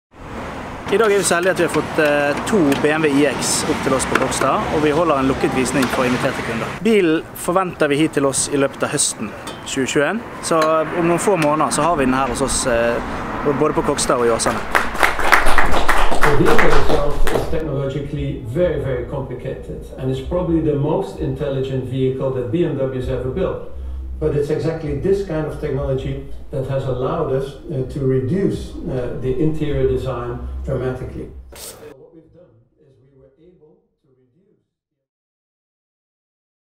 Sneak peek på nye BMW iX
Bli med til Jæger Sentrum på Kokstad og inn på en liten smugtitt på nye elektriske BMW iX.

Produsert av RUCK.no